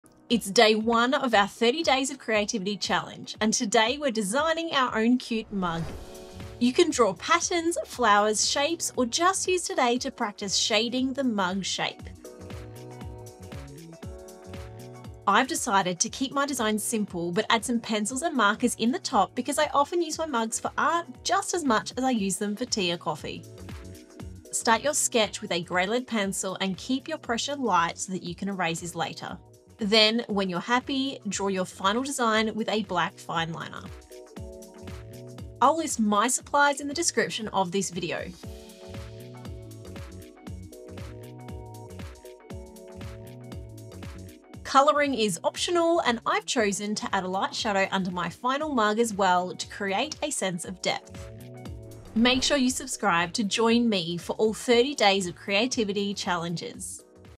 Day 1: How to draw a mug with pencils - 30 Days of Creativity (Johanna Basford)
It’s Johanna Basford’s 30 Days of Creativity – Day 1!

Today’s challenge was to draw your own mug pattern. With the mug outline already done, you can keep it simple or get as creative as you want. I decided to keep the actual mug simple, but added some markers and pencils in the top!

Supplies Used:
- Greylead pencil
- Eraser
- Staedler Pigment Liners 0.7mm and 0.3mm
- Red 885 Tombow Dual Brush Pen
- Shading: Prismacolor pencils(1059 & 936)
- Pens & pencils in mug: Prismacolor pencils (921, 1030, 935, 903, 923, 917, 1011, 905) and Tombow Dual Brush 703

- 30 Days of Creativity by Johanna Basford
- Prismacolor Pencils
- Tombow Dual Brush Pens

This book was given to me by @johannabasford for this challenge.